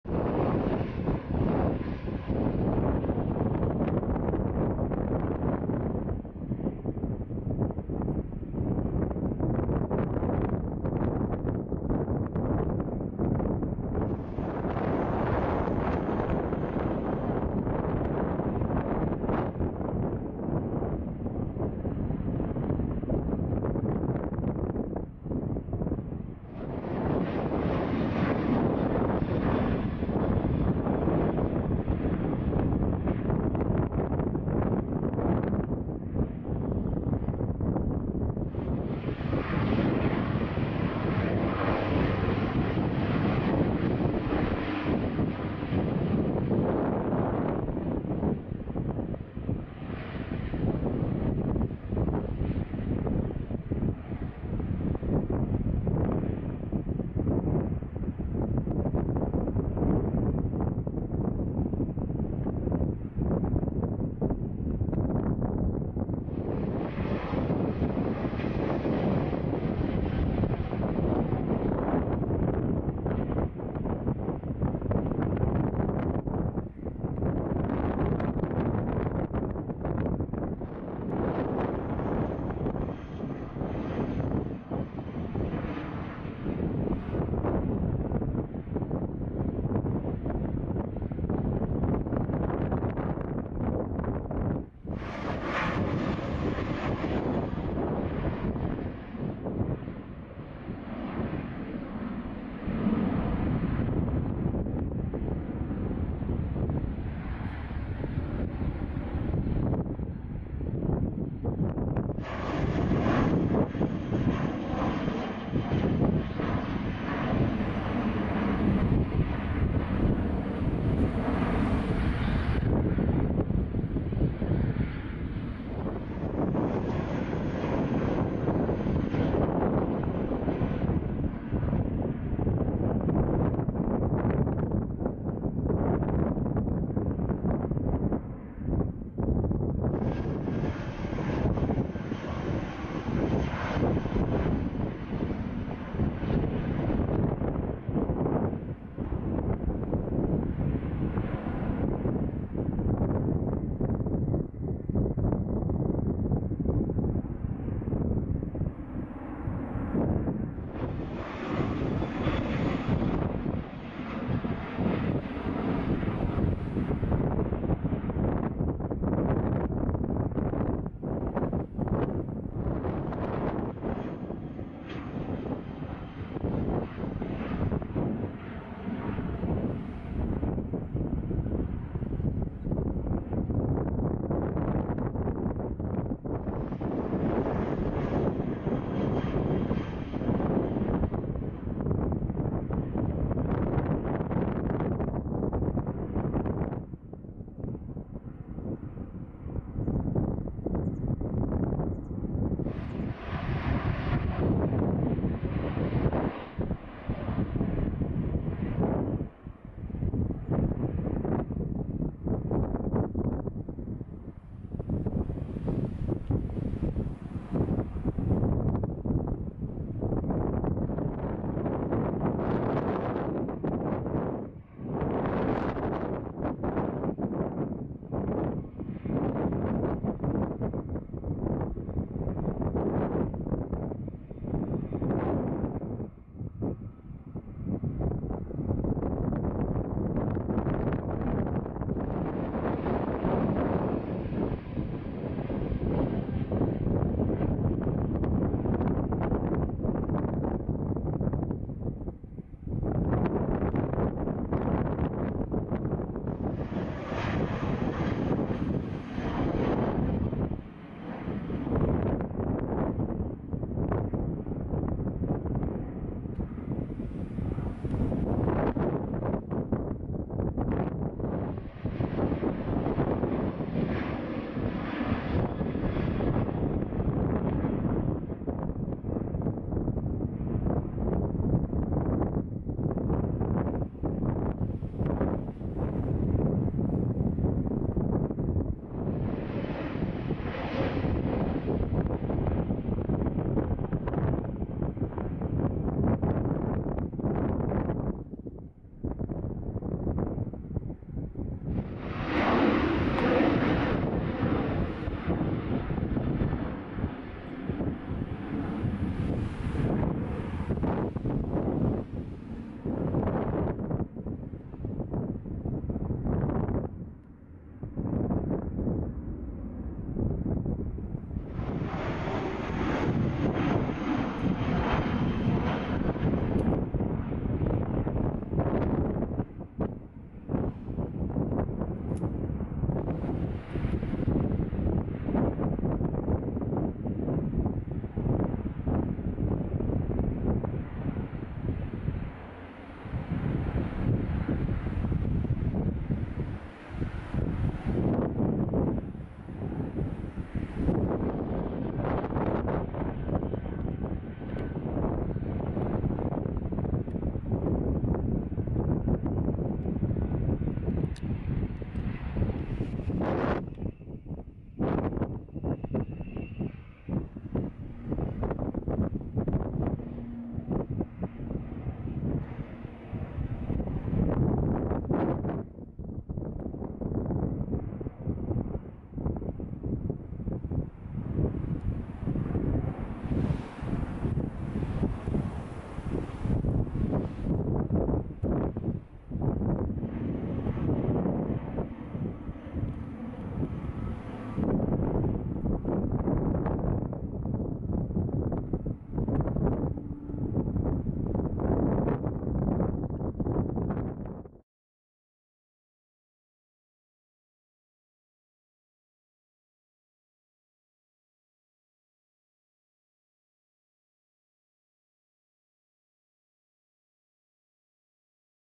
Plane Spotting Athens (ATH) | Windy Morning Spotting | October 2021 (Part1)
Welcome to the channel! in this video you will see some windy plane spotting forom Athens International Airport (ATH), in Greece.